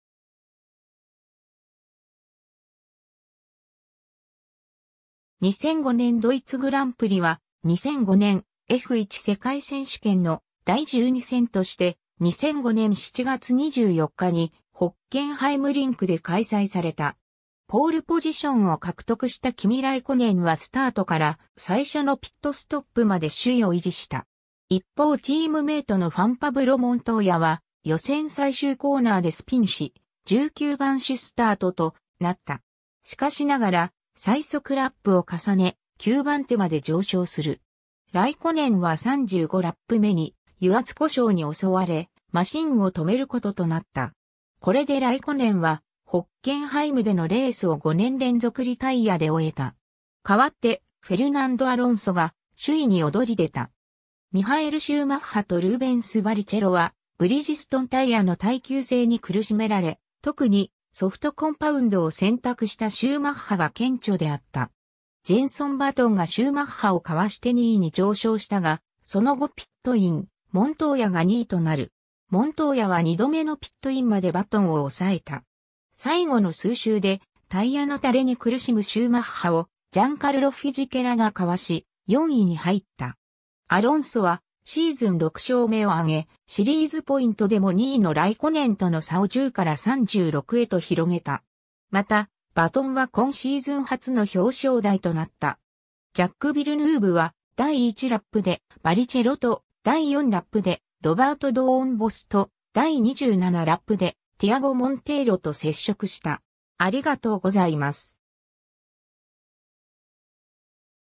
2005年ドイツグランプリ
バーデン＝ヴュルテンベルクのスポーツ競技大会
フェルナンド・アロンソ
2005年ドイツグランプリ (LXVII Groser Mobil 1 Preis von Deutschland) は、2005年F1世界選手権の第12戦として、2005年7月24日にホッケンハイムリンクで開催された。
 ポールポジションを獲得したキミ・ライコネンはスタートから最初のピットストップまで首位を維持した。
一方チームメイトのファン・パブロ・モントーヤは予選最終コーナーでスピンし、19番手スタートとなった。
しかしながら最速ラップを重ね、9番手まで上昇する。
 ライコネンは35ラップ目に油圧故障に襲われ、マシンを止めることとなった。
これでライコネンは、ホッケンハイムでのレースを5年連続リタイヤで終えた。
代わってフェルナンド・アロンソが首位に躍り出た。
ミハエル・シューマッハとルーベンス・バリチェロはブリヂストンタイヤの耐久性に苦しめられ、特にソフトコンパウンドを選択したシューマッハが顕著であった。
ジェンソン・バトンがシューマッハをかわして2位に上昇したが、その後ピットイン、モントーヤが2位となる。
モントーヤは2度目のピットインまでバトンを抑えた。
最後の数周でタイヤのたれに苦しむシューマッハをジャンカルロ・フィジケラがかわし、4位に入った。
 アロンソはシーズン6勝目を挙げ、シリーズポイントでも2位のライコネンとの差を10から36へと広げた。
また、バトンは今シーズン初の表彰台となった。
 ジャック・ヴィルヌーヴは第1ラップでバリチェロと、第4ラップでロバート・ドーンボスと、第27ラップでティアゴ・モンテイロと接触した。